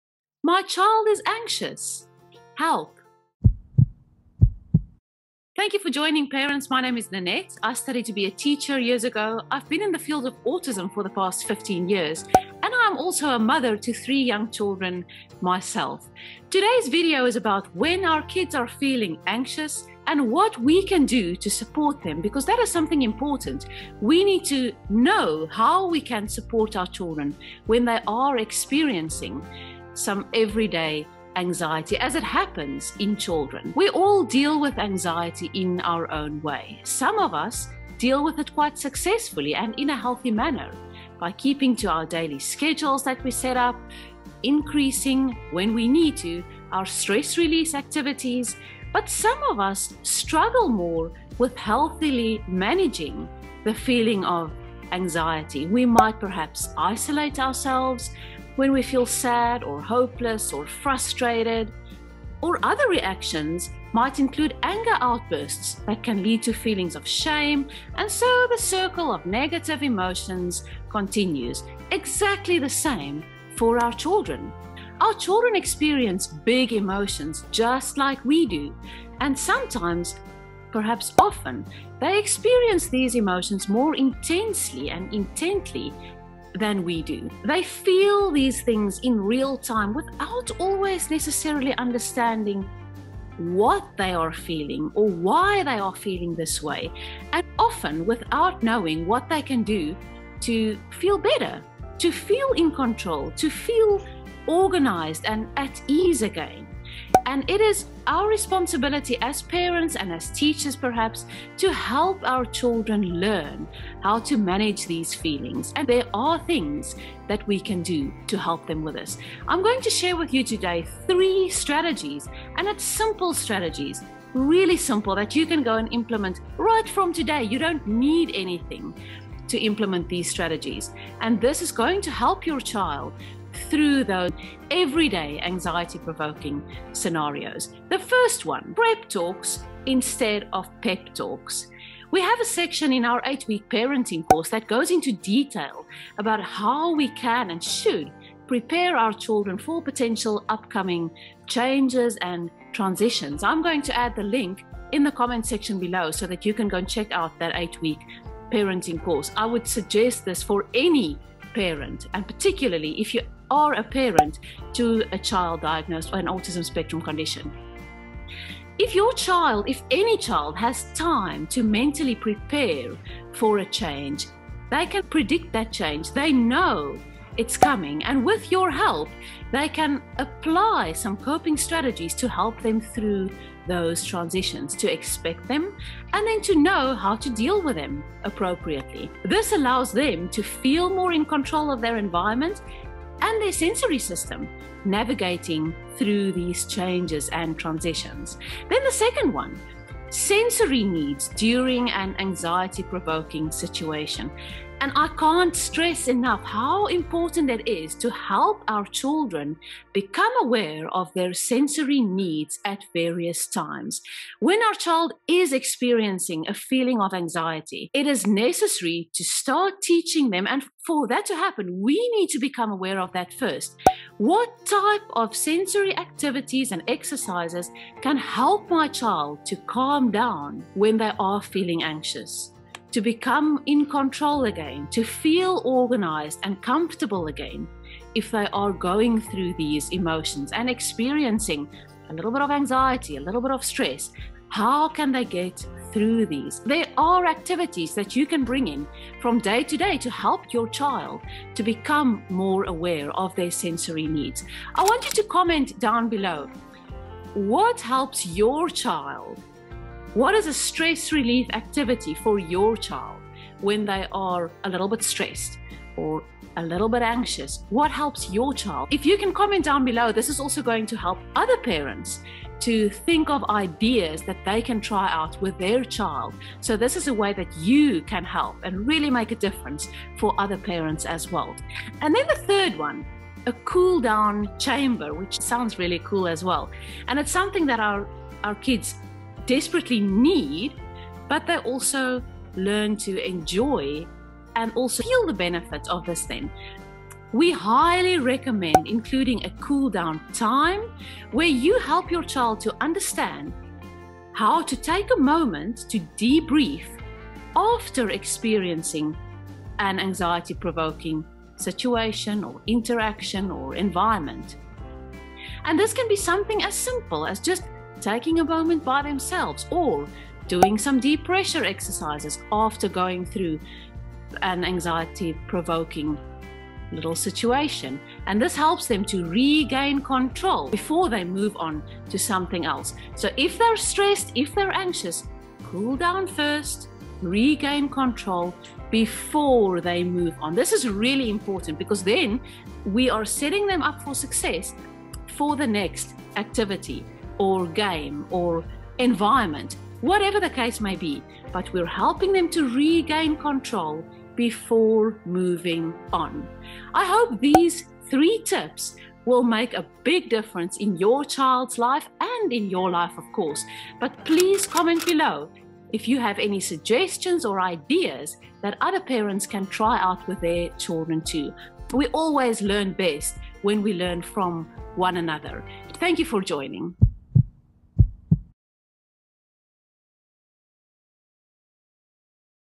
Anxiety In Children - Support YOU Need To Hear!
We all deal with anxiety in our own way. Some of us quite successfully in a healthy manner, by keeping to our daily schedules and increasing, when needed, our stress-relief activities. Some of us struggle more with healthily managing our anxieties and might isolate ourselves when we feel sad, hopeless, or frustrated. Other reactions might include anger outbursts that can lead to feelings of shame and so the circle of negative emotions continues.

Similarly to this, our children feel “big” emotions just as or sometimes more intensely than us. They feel these in real-time, without always understanding the actual emotion, that it will most probably pass or diffuse with time and that there are certain activities we can practice to speed this process along.

So what can we do as parents to support our children with their inevitable feelings of anxiety?

*Prep talks instead of pep talks - we have a section in our parent course that goes into detail about how important preparing any child for potential upcoming changes and transitions is. If our child has time to prepare for a change mentally, they can predict (with your help) various scenarios. This allows them to feel more in control of their environment and sensory system navigating through these.

*Sensory needs during anxiety-provoking situations. I can’t stress enough how important it is to help your child become aware of his or her sensory needs at various times. When he is feeling anxious - which activities tend to calm him, when he is feeling lethargic - which activities tend to excite him, etc. The most appropriate way to help here is to ask advice from a sensory integration specialist, but there are activities you can bring in from today to help your child become more aware of these needs.

*Cooldown chamber - we highly recommend including a “cool down” time where you help your child understand that taking a moment to debrief and de-escalate is needed and healthy after a possible anxiety-provoking interaction or environment.

We learn best when we learn from each other.

Follow this link for more information on a new and exciting 8-week parenting course:

And another great video where Dr. Temple Grandin spoke about being autistic and the anxiety she experienced:

Timestamps:
0:00 Anxiety in children
1:59 Three strategies to help your child
2:18 Prep talks instead of pep talks
3:19 Sensory needs
4:43 Cool down chamber
5:55 Comment to help other parents